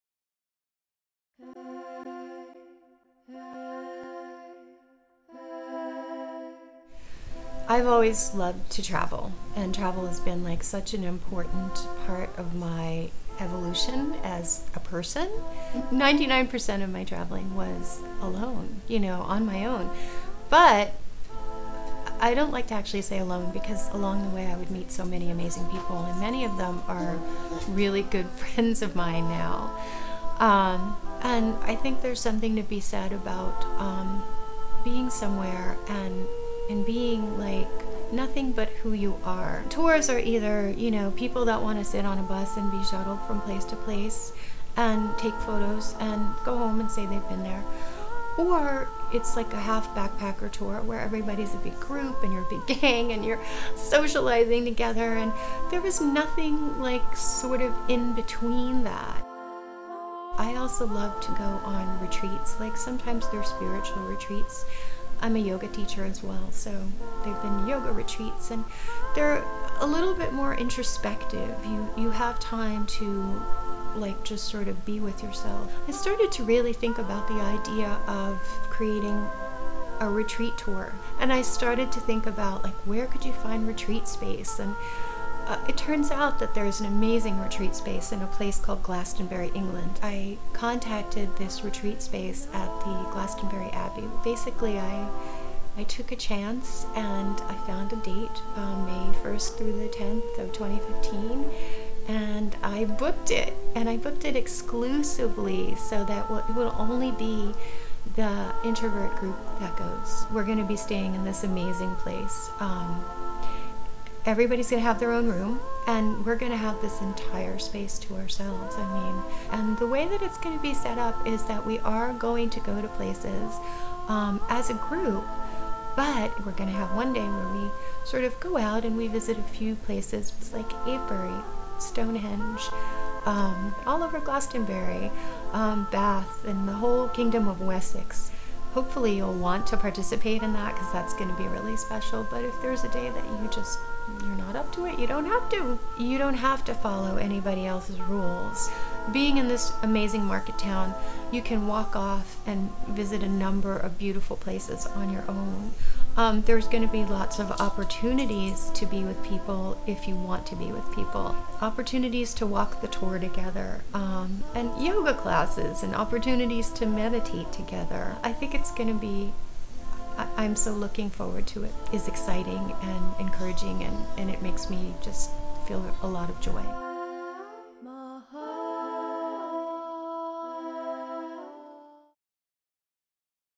Sacred Introvert Retreat Tour: Details
"The First Travel Experience for Introverts by Introverts"
May 1~10 of 2015 traveling to Mystical Glastonbury England and beyond. The Somerset/Wessex region of England is an area Lisa Avebury, Sacred Introvert's founder and creator, knows well. She made her first trip there, traveling on her own, in 1995 and has been going back regularly ever since. This area of England, surrounded by green countryside, is dotted with ancient and mystical sites that fire the imagination and feed the soul. For full details and booking Early Birds save $300. before Nov. 30th.